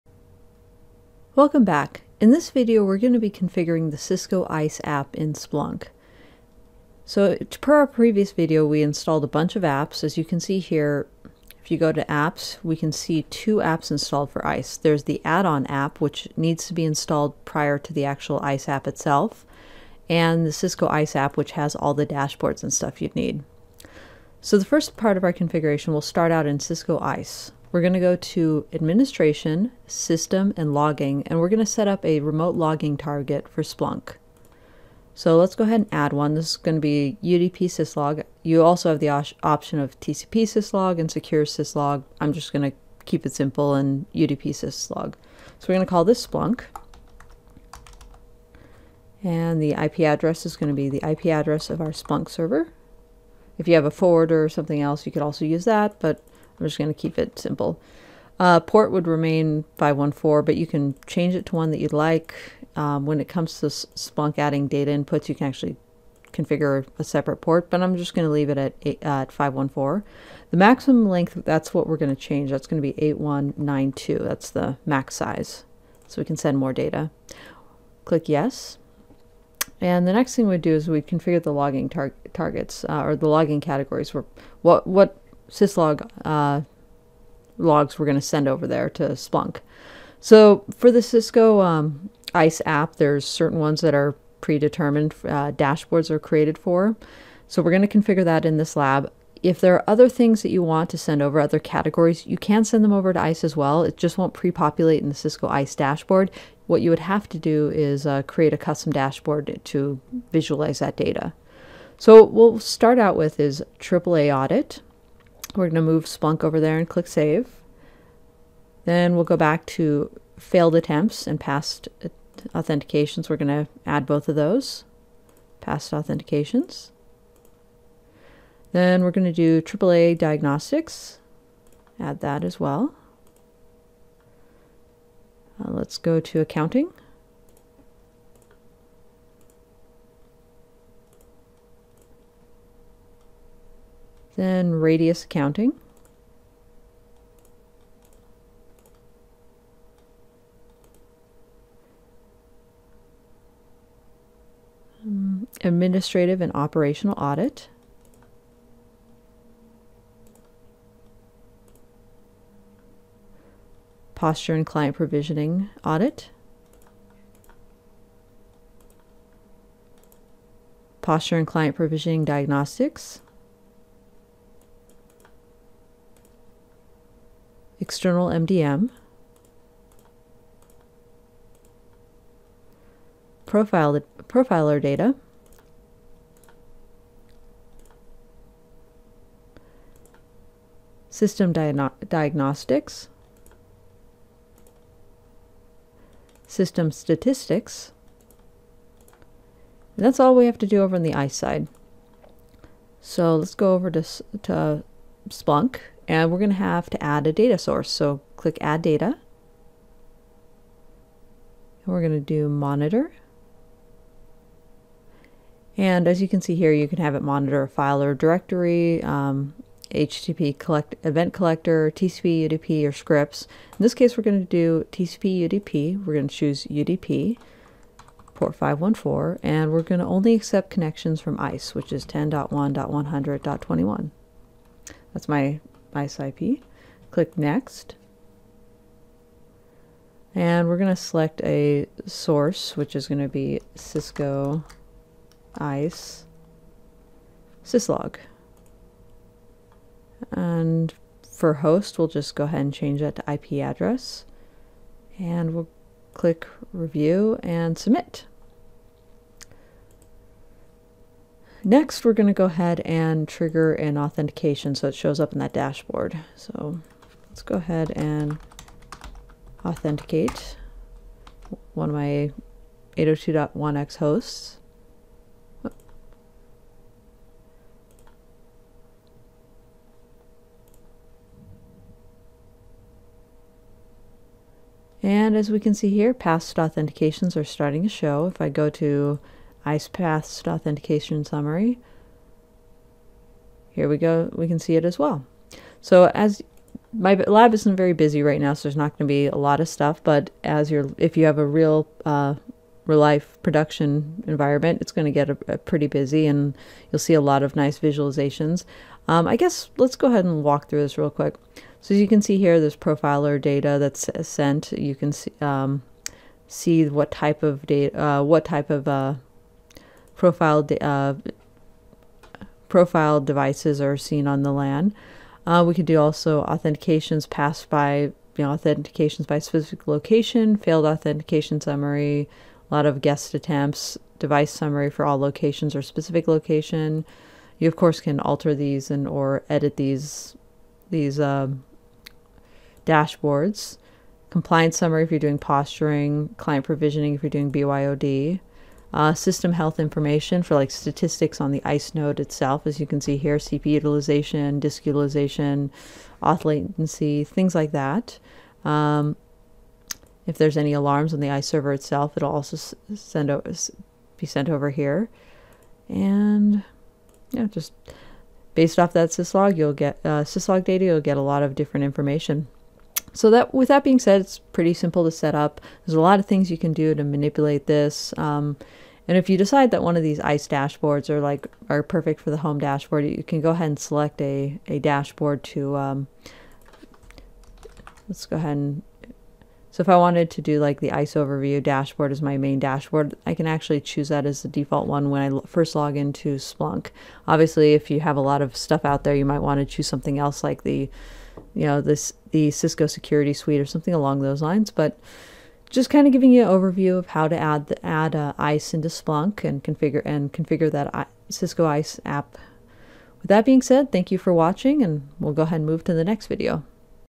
Configuring the Cisco ISE App for Splunk
In this video, we’ll be configuring the ISE app in Splunk. We’ll configure ISE to send syslog data to Splunk and configure the data input in Splunk. After that has completed, we will verify that the data is populating in the Splunk ISE app dashboard.